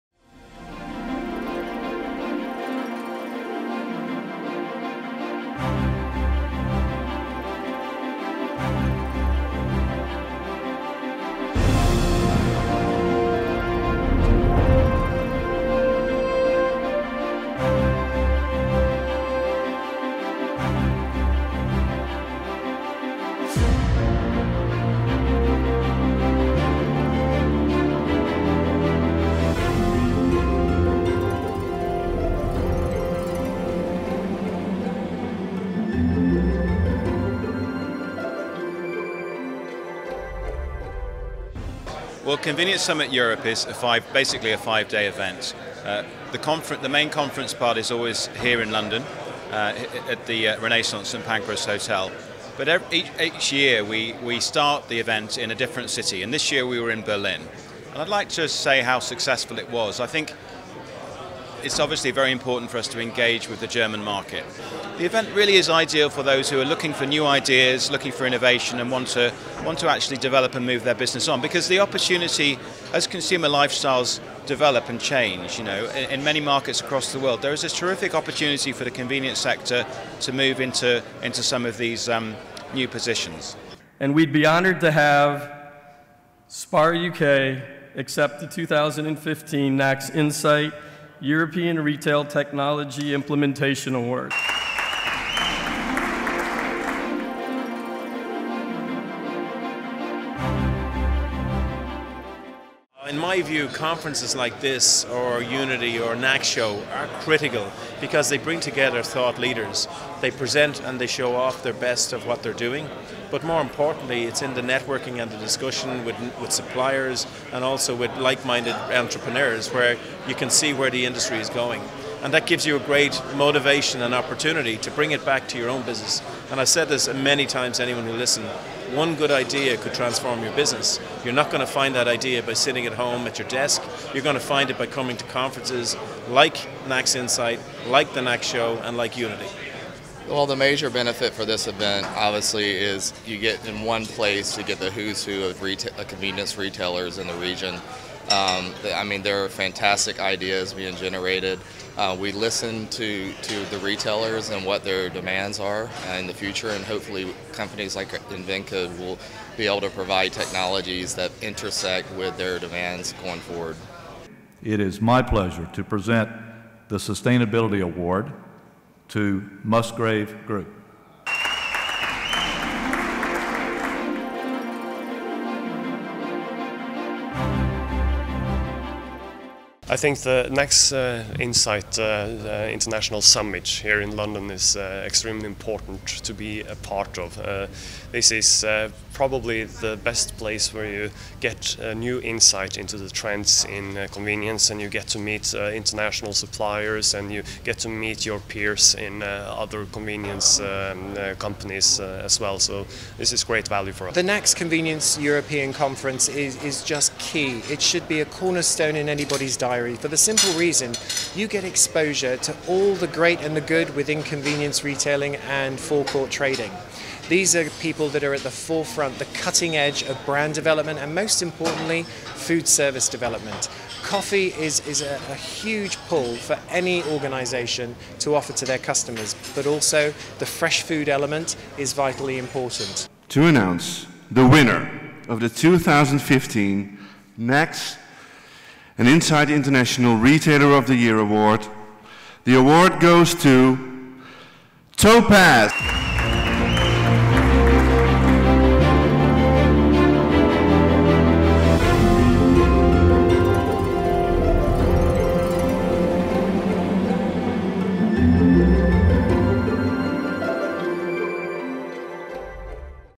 Video report - NACS Insight Summit Europe 2015
The PetrolPlaza team attended this year´s NACS Insight Convenience Summit – Europe, which brought together convenience and fuel retailers from around the world to Berlin and London.